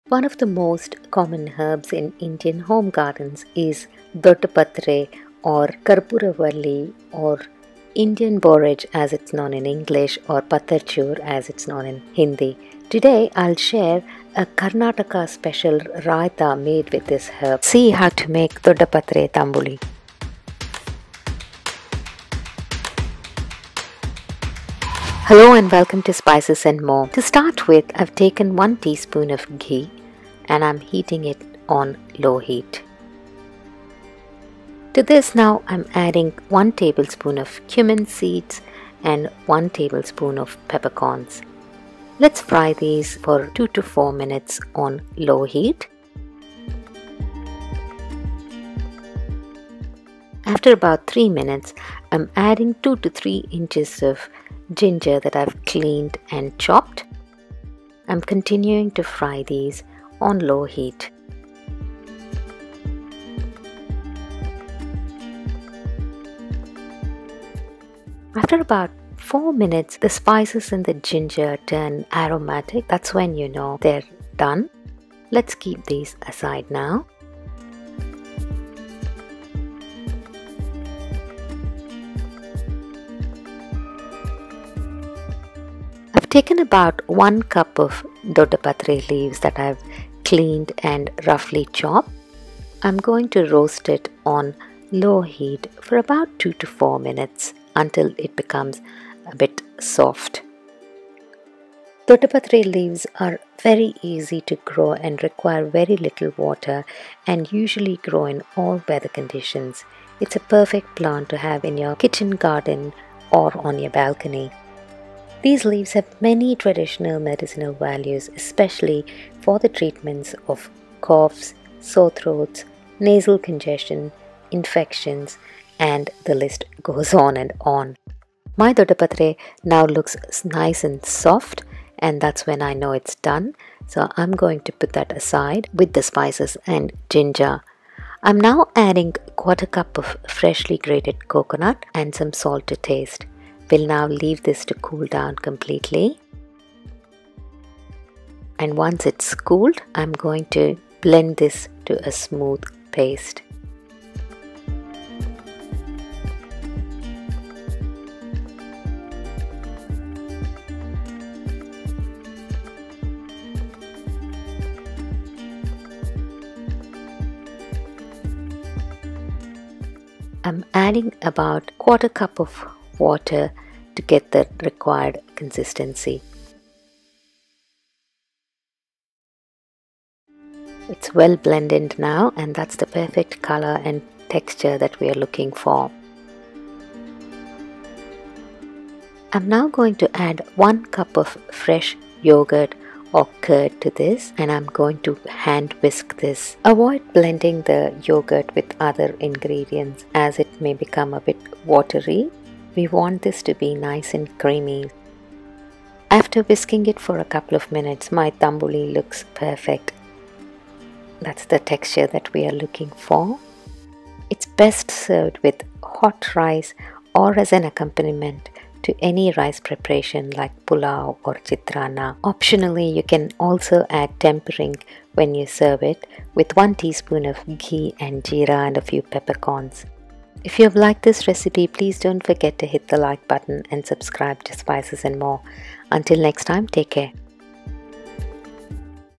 doddapatre tambuli|karpurvalli raitha|ದೊಡ್ಡಪತ್ರೆ ತಂಬುಳಿ
Doddapatre is a common herb grown in Indian kitchen gardens. This herb has a unique taste and many medicinal benefits. Todays recipe is a yoghurt based speciality from Karnataka commonly known as tambuli. Try this awesome recipe with plain rice or as an accompaniment to a rice preparation.